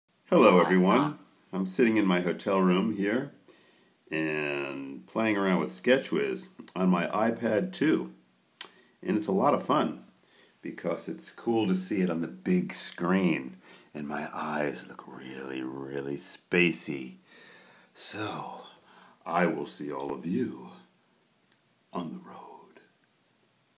SketchWiz on the IPad
Trying out SketchWiz on my IPad 2.  Really fun!